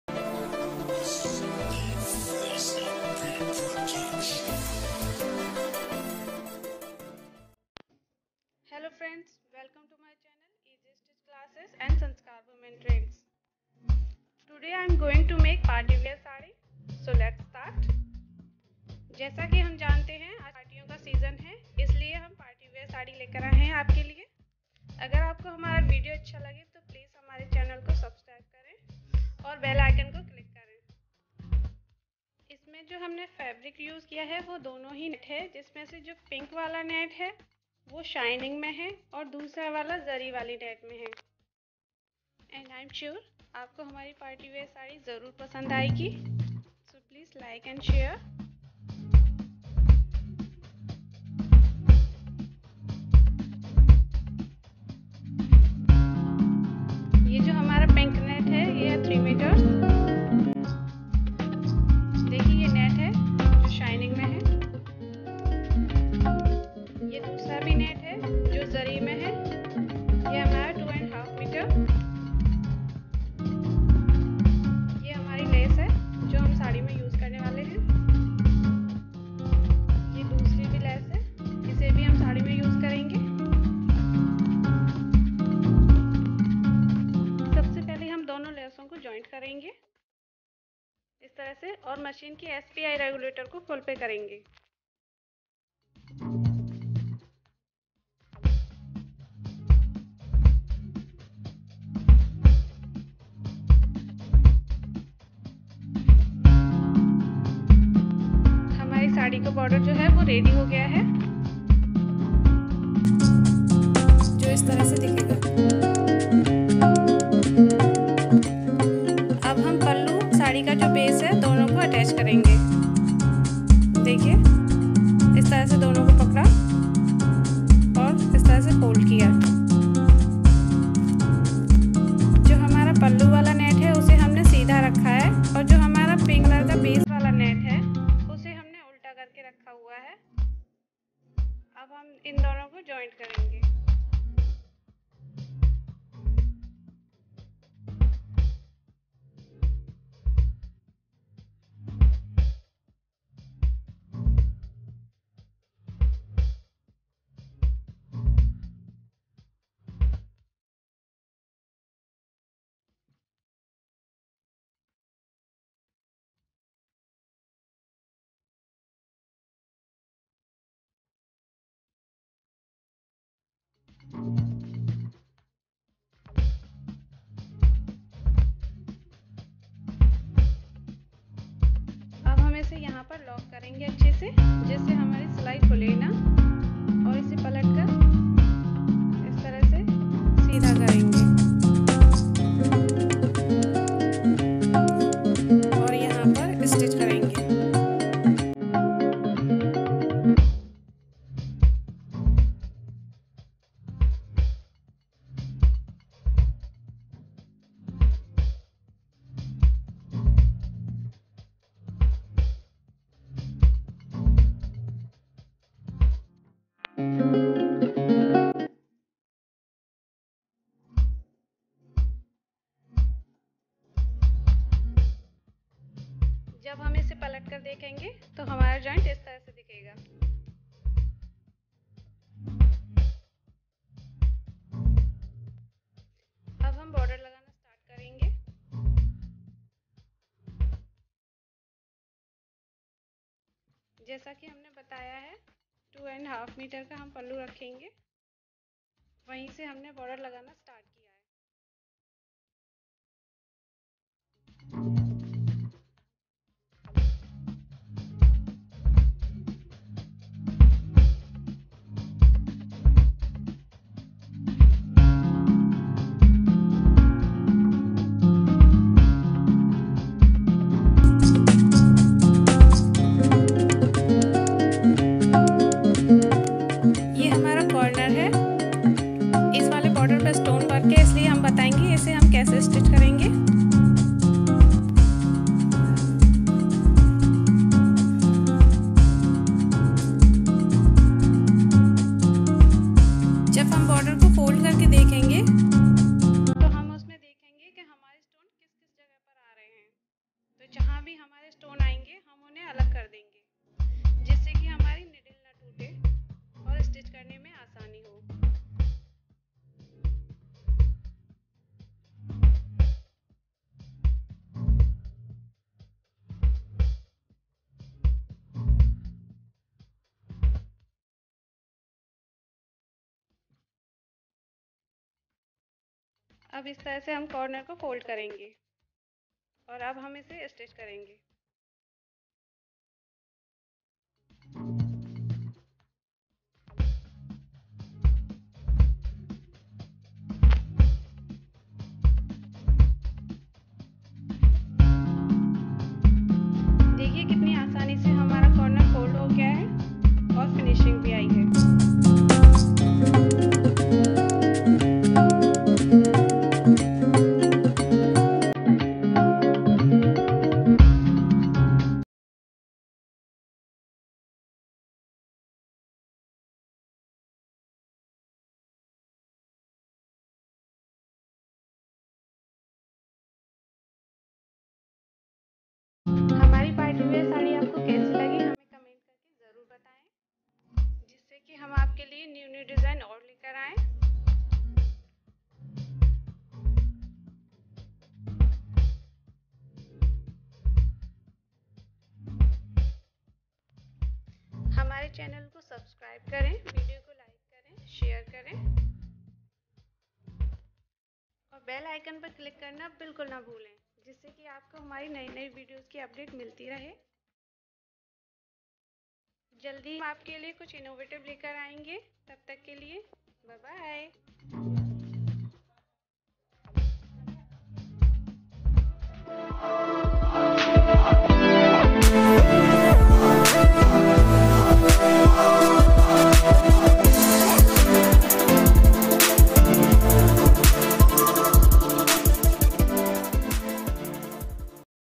Designer Saree || Step by step Tutorial | How to Make Party Wear Saree || Party Wear Sarees
Make Party Wear Saree || Designer Saree | Party Wear Sarees | Step by step Tutorial
 Try My this Tutorial Make Your Own Designer Saree At Home  and Look Gorgeous,In Easy stitch classes You would find all Designer Work Stitching,which might Cost you ,follow my tutorials and come up with Party Wear Sarees and much more

Regards
Easy Stitch Classes
Mausam Singh